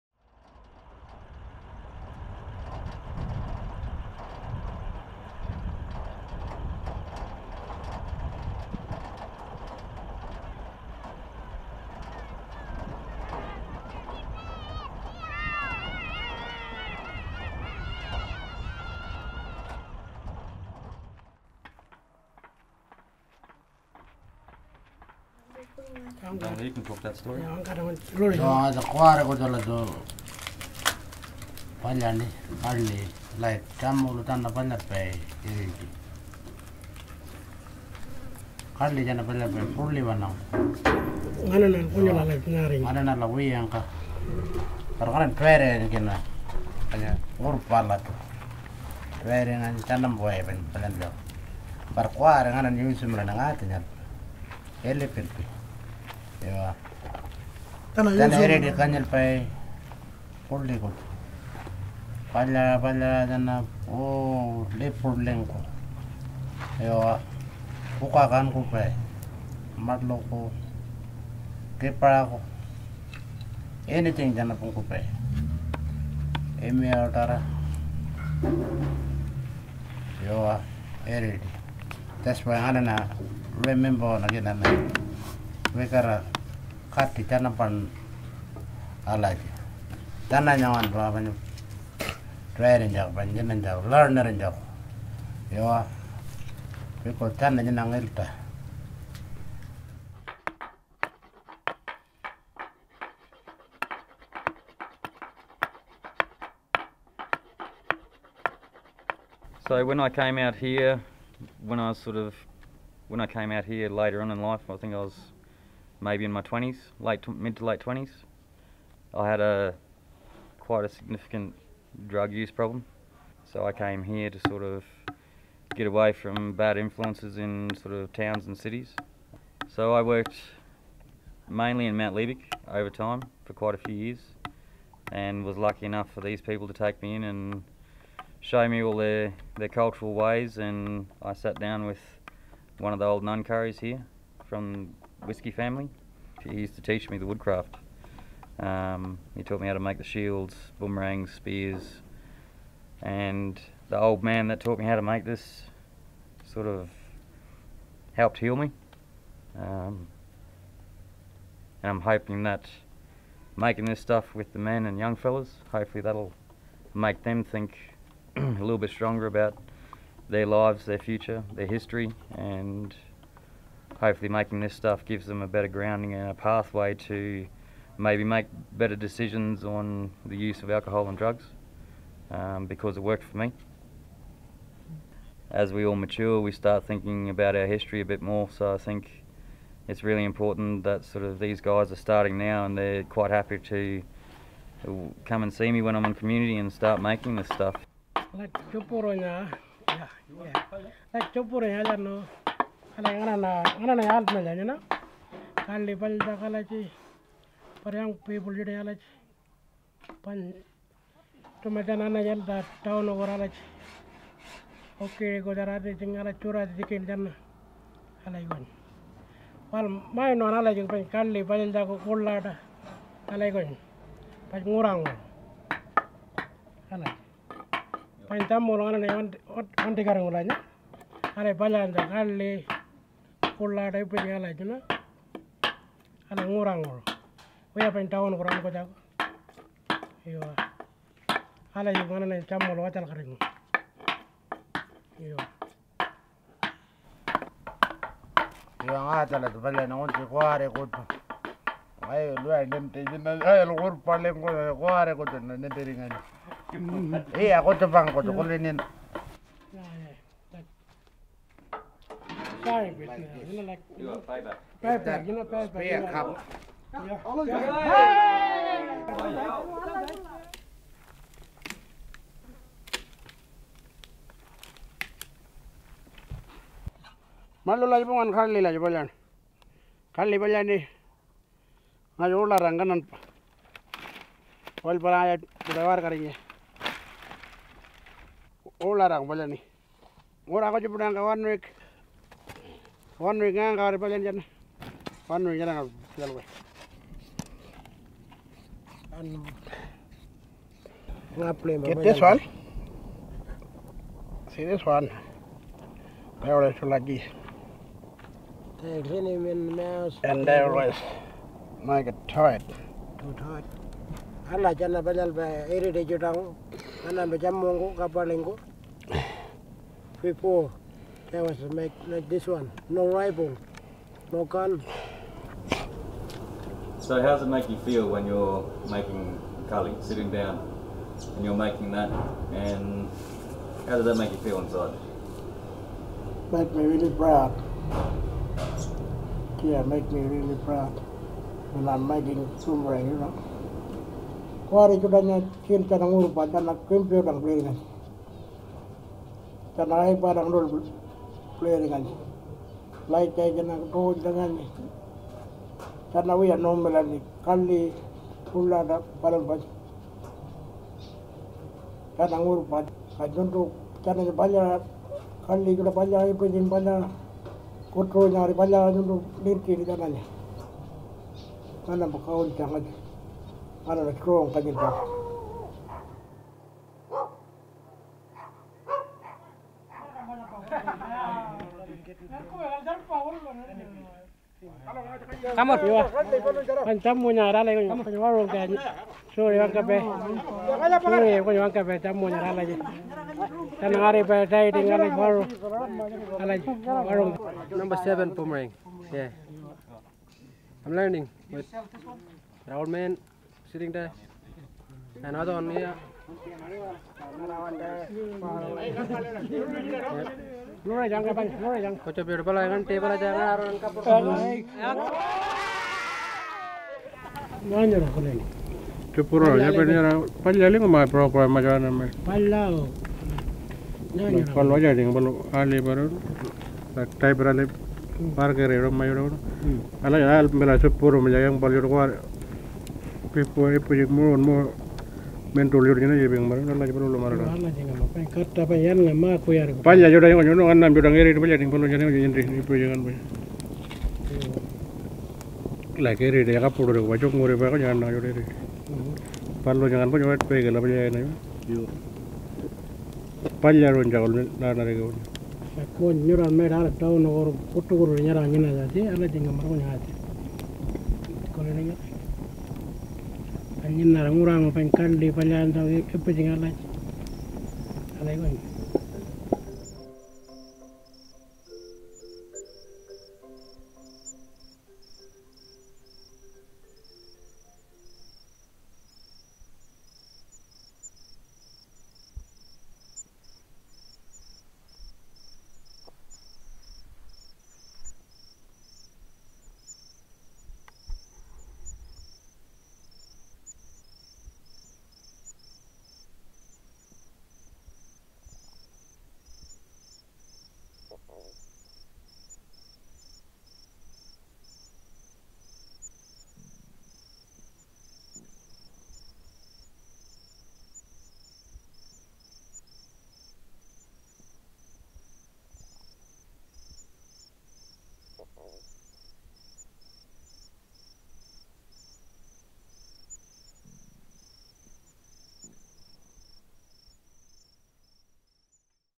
CASSE Men's Tjilirra Movement. Changing minds, saving lives.
The Men’s Tjilirra Movement was established to secure the future for the custodians of the world’s longest continuing culture.
Tjilirra - traditional handmade tools including boomerangs, shields, spears, carrying vessels - are the cornerstone of the ancient traditional world of the Pintupi men. Sacred traditional tool carvings hold the power to the country. Tjukurrpa (the Dreaming)
holds the meaning and the life for them, reinforcing relatedness, sorrow and country.
Tjilirra represents ancient law and land-ownership. The carvings hold the journeys of ancestral beings and sacred places in the landscape and more. The Tjilirra is the title or deed for the country of the men. Tjilirra is the power for the men.
The heart of the Men’s Tjilirra Movement lies in Australia’s remote central and western desert region, in the communities of Haasts Bluff (Ikuntji), Mt Liebig (Watiyawanu), Kintore (Walungurru), Papunya (Warumpi) and Kiwirrkurra.